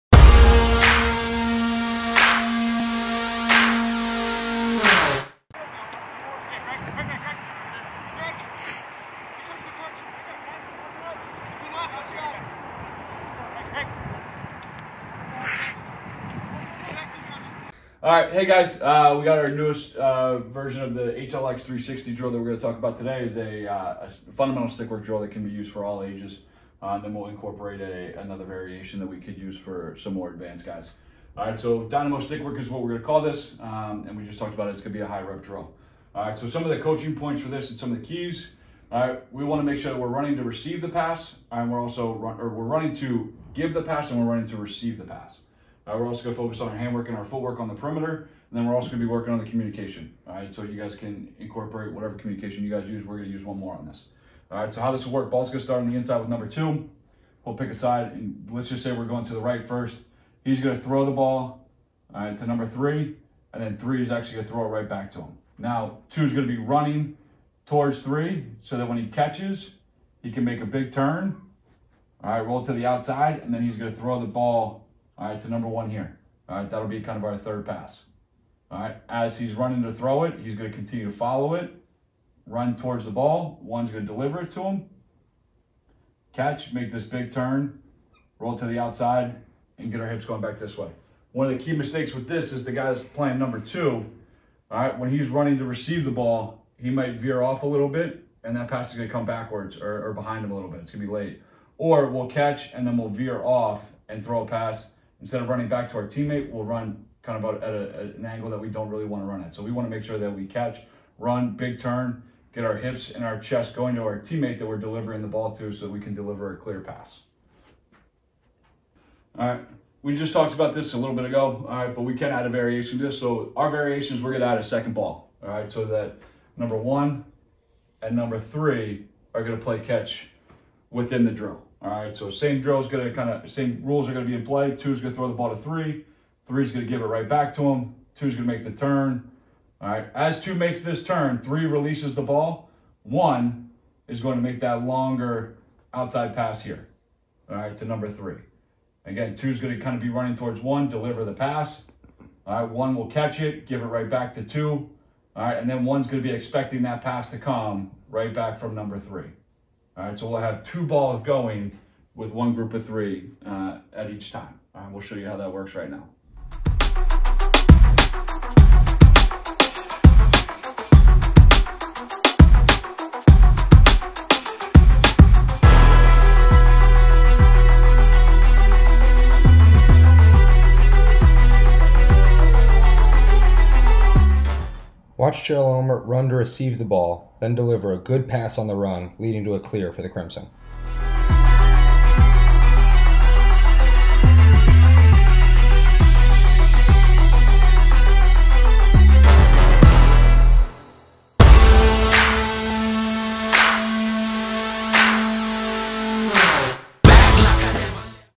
HLX360°: Dynamo Passing
On this episode, Coach Hutchinson teaches a great stickwork work that particularly helps in the transition game